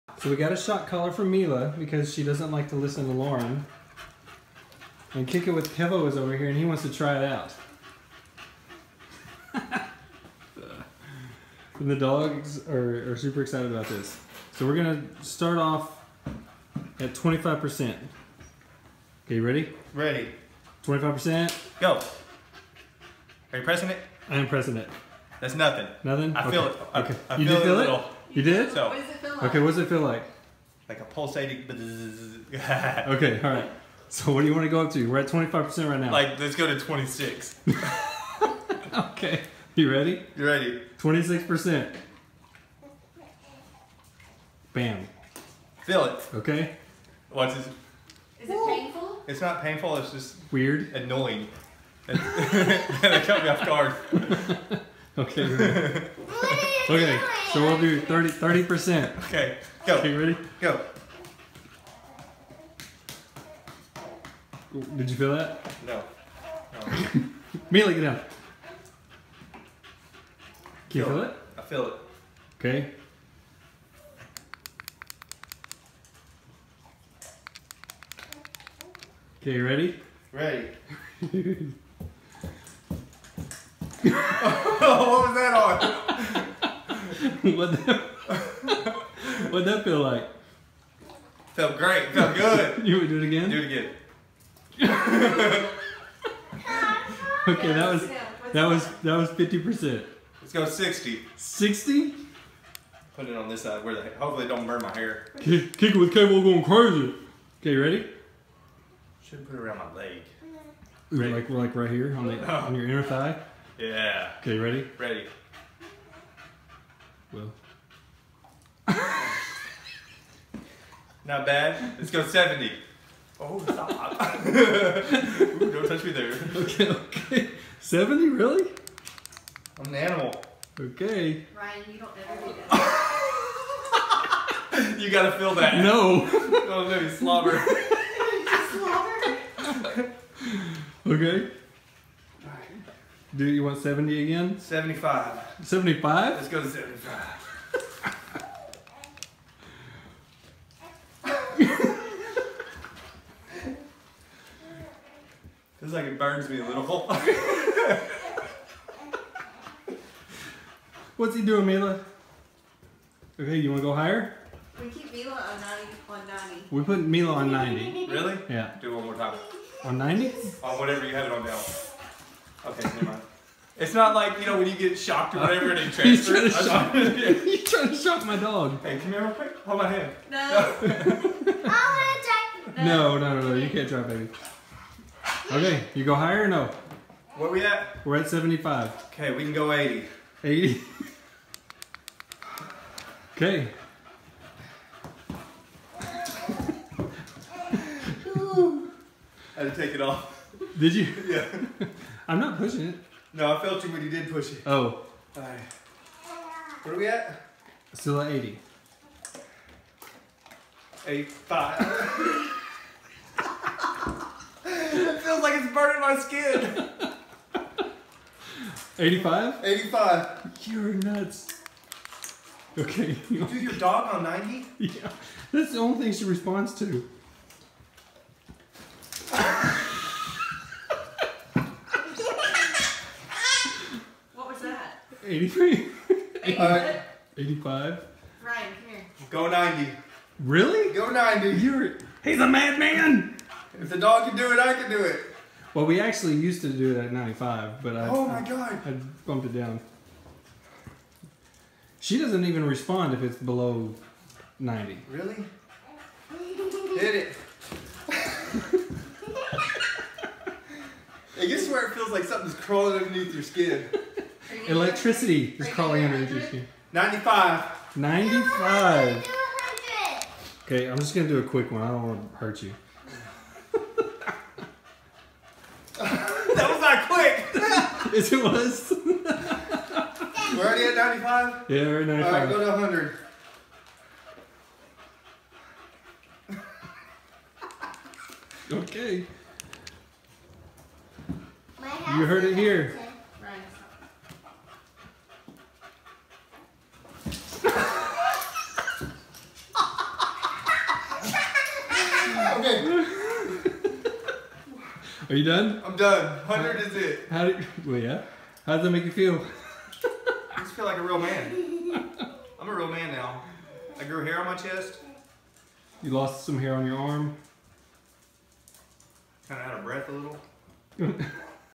Guy tries on a shock collar
Kickitwithkevo tries out my dogs shock collar